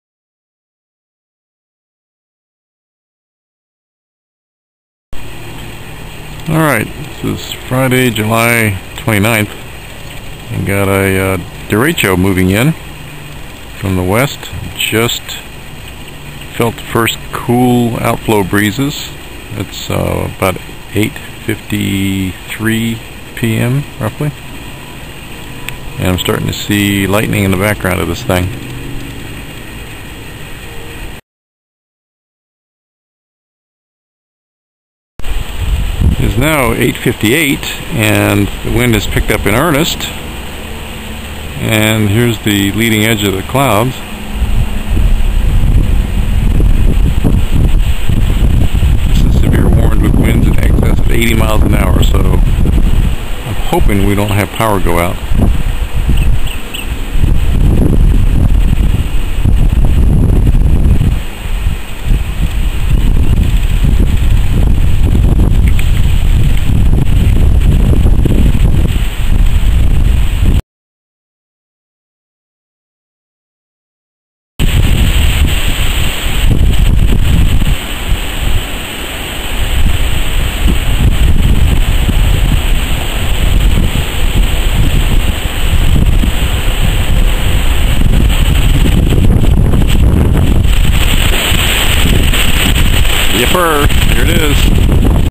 June 29 2012 Derecho approaching the Roanoke Valley of Virginia
I was outside my house videoing this complex as it approached.  I had to go inside after the first real gust almost blew me off my feet.